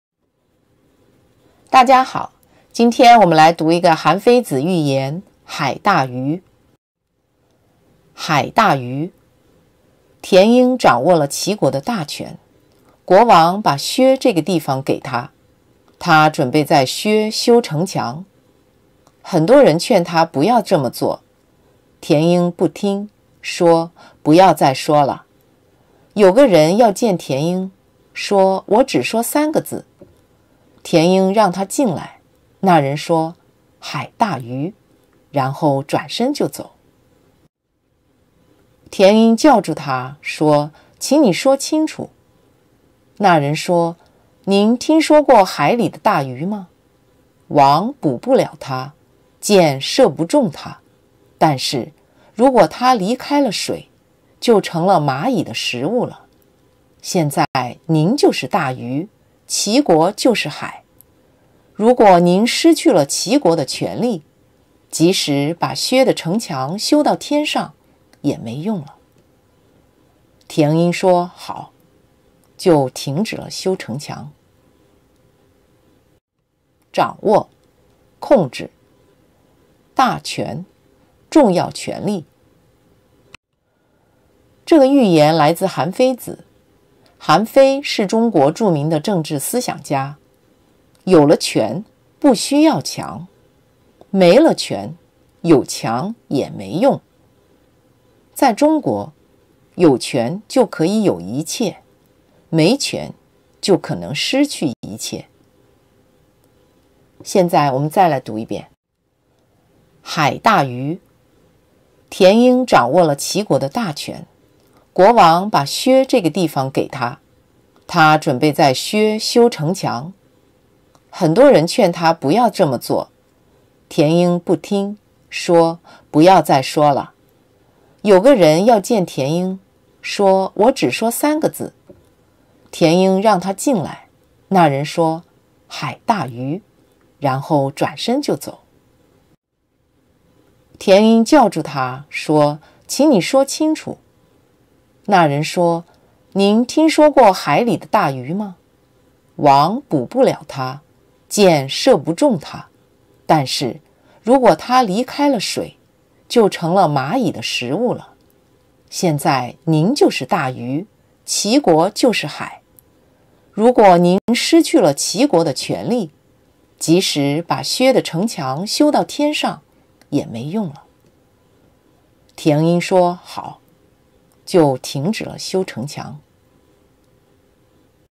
【韩非子寓言】海大鱼 Big Sea Fish【Hanfeizi Fable】
海大鱼
韩非子

Big Sea Fish
Hanfeizi

田婴掌握了齐国的大权，国王把薛这个地方给他。他准备在薛修城墙，很多人劝他不要这么做，田婴不听，说：“不要再说了。”

Tian Ying held the power of Qi, and the king gave Xue to him. He wanted to build walls in Xue. Many people tried to dissuade him from doing this, but Tian did not listen and said, “Don’t talk about this anymore.”

【中国经典故事 Classic Chinese Story】